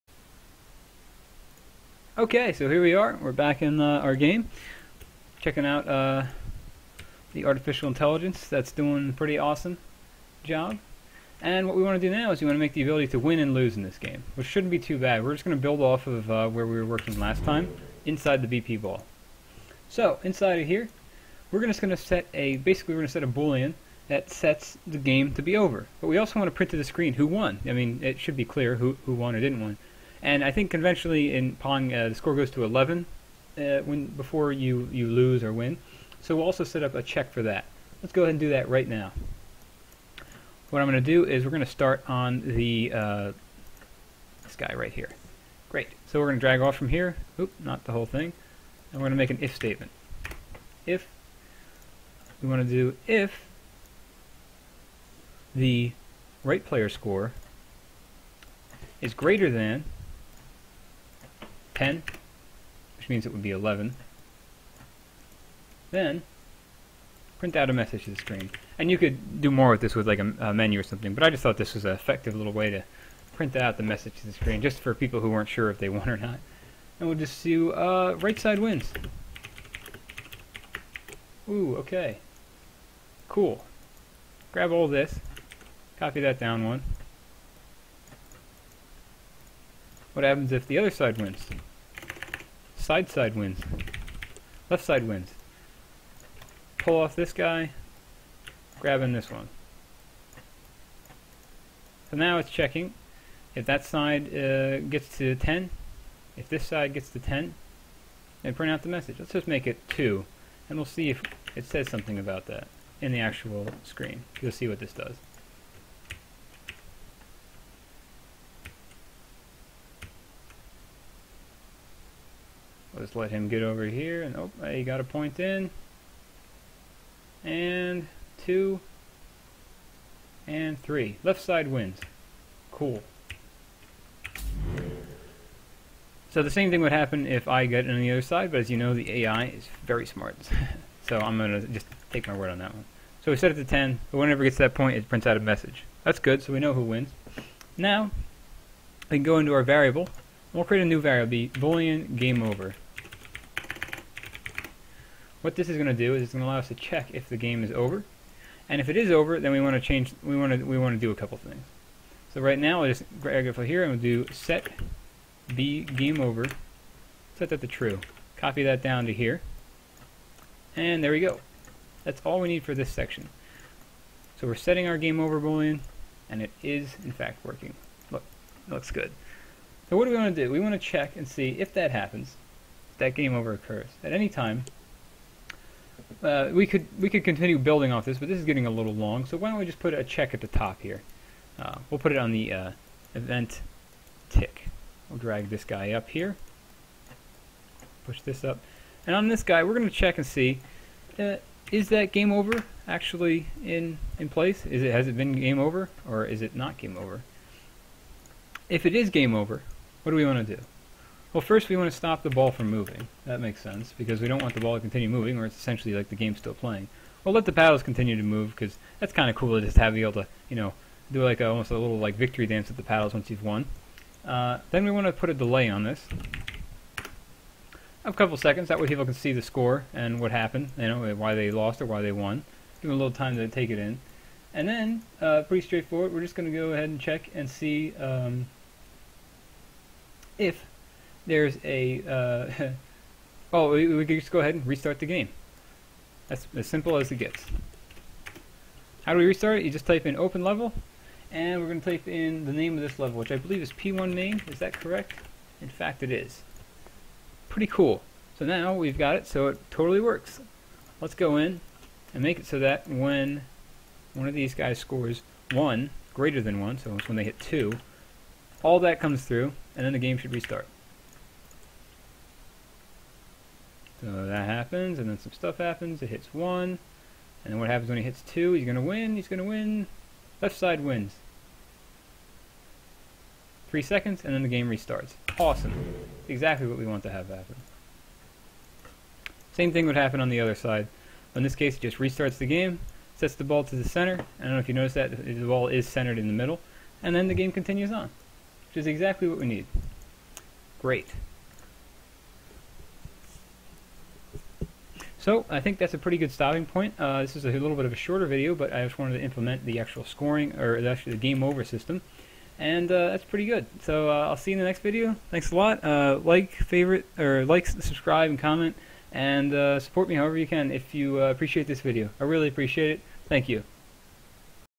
How to Make Pong In UE4 | Part 10 | Win/Lose + Restart Level
In this tutorial series I teach you how to create a clone of "Pong" in Unreal Engine 4, using Blueprints.

The series will cover everything you need to know from creating meshes, scripting functionality, setting up menus, adding audio, loading levels, creating menus, and deploying your final game.

Plus a few tips and tricks along the way!

I hope this tutorial is helpful to anyone interested in learning more about Unreal Engine 4.

If you found this helpful if so please let me know with your support!

GameUserSettings.ini:

[/Script/Engine.GameUserSettings]
bUseVSync=False
ResolutionSizeX=1200
ResolutionSizeY=900
FullscreenMode=2
WindowPosX=-1
WindowPosY=-1
Version=5